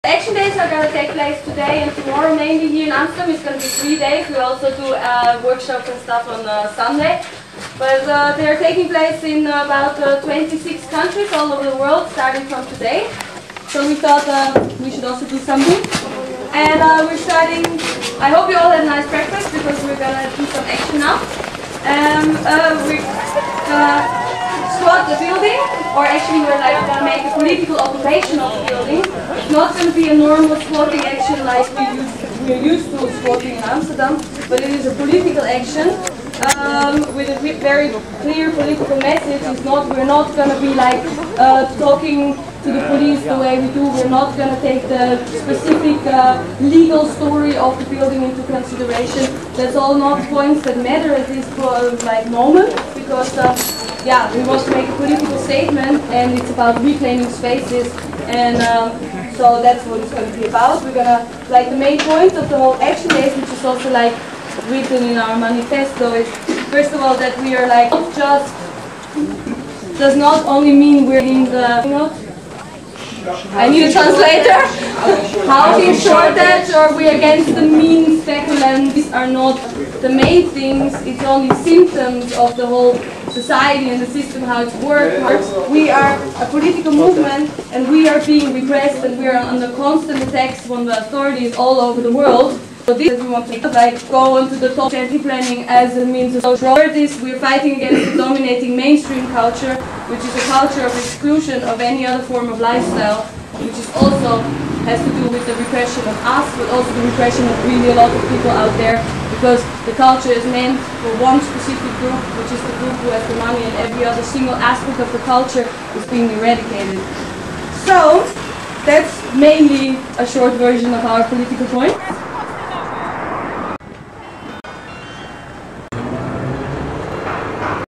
Squatting of Rijks Museum
This is the next little bit more edited version off squatting the Rijks museum (tekenen school) Soon would come more